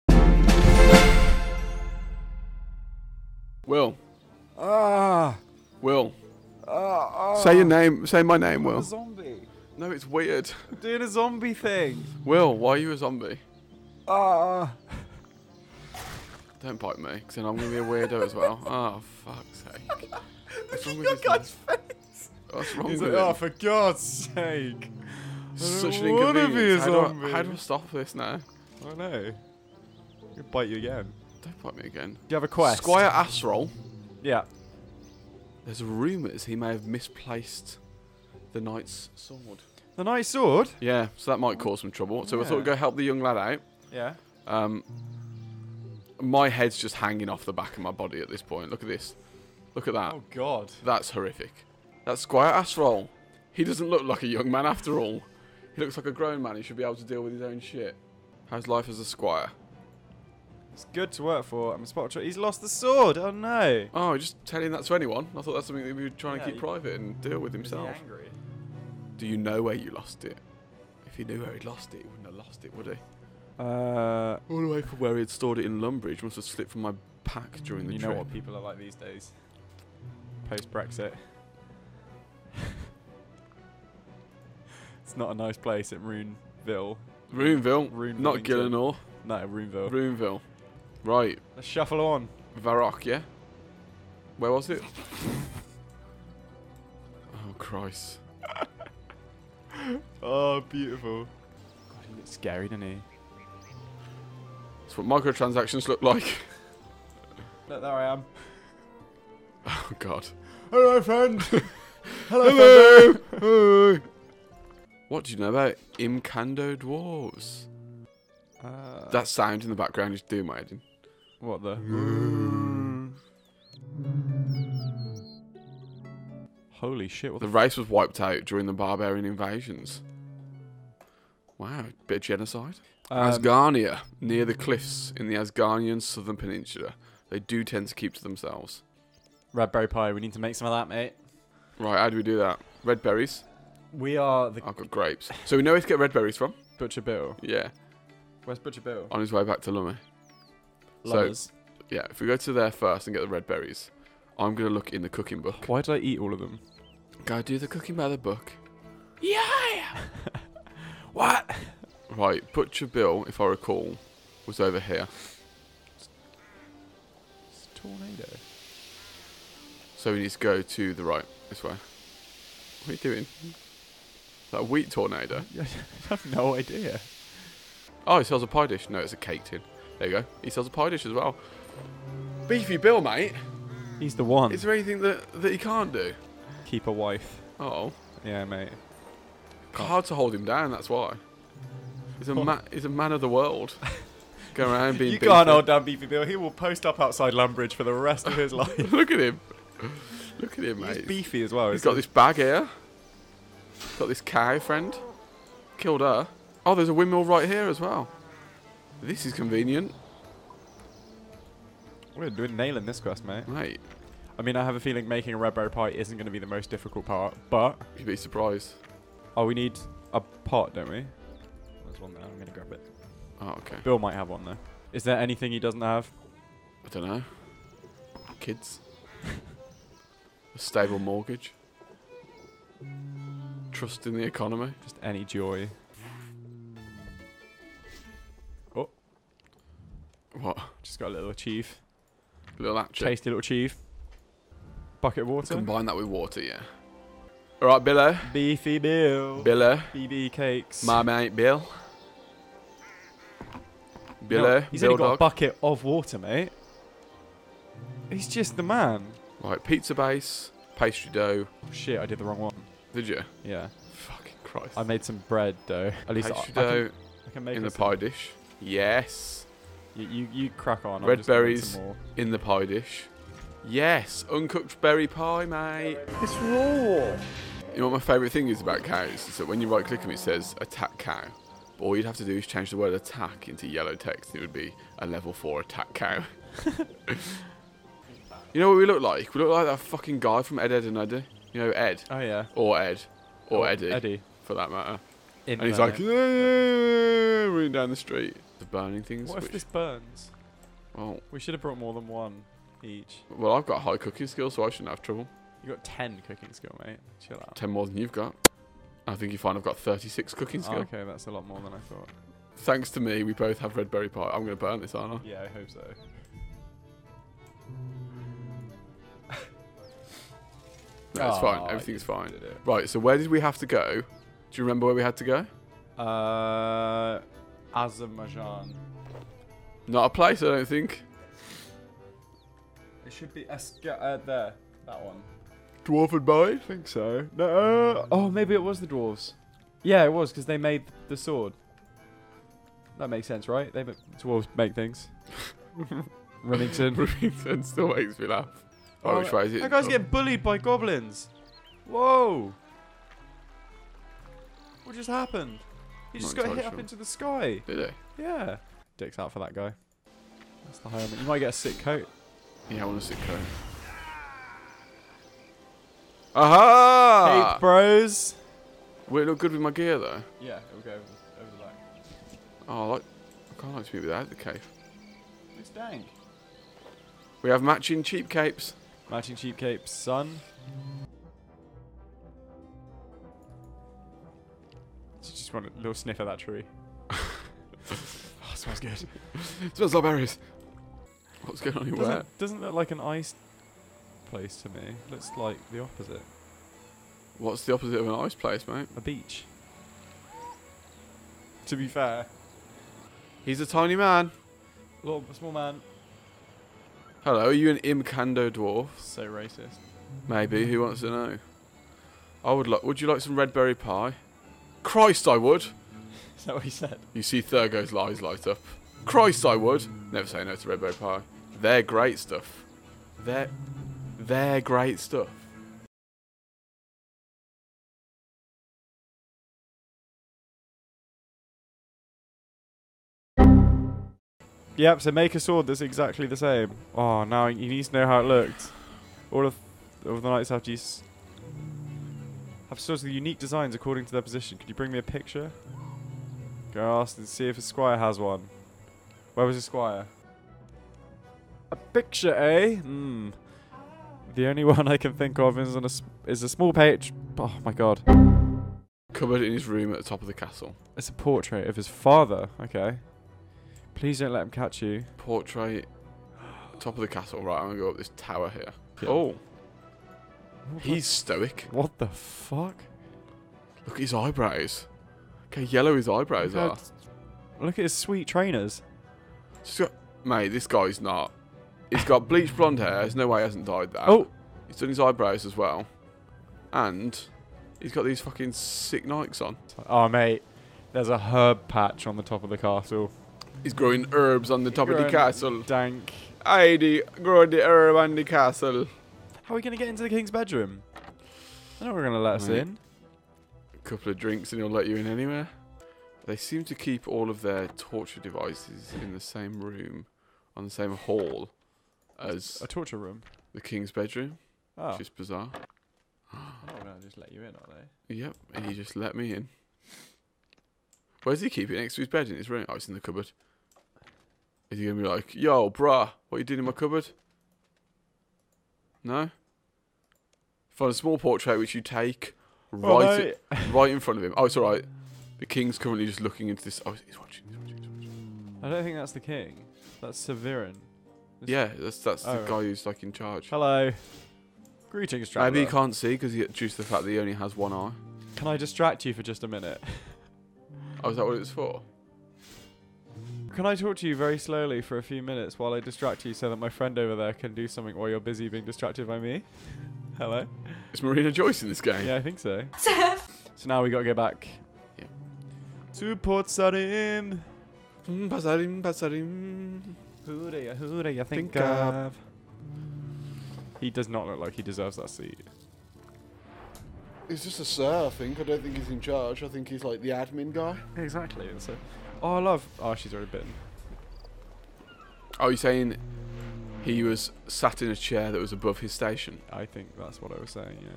Butcher, Baker, Sword-Maker? | The Always Saga EP 11
Our heroes are reuinited with a legendary trader as they revisit old locations and old friends in the hope of helping a distressed squire.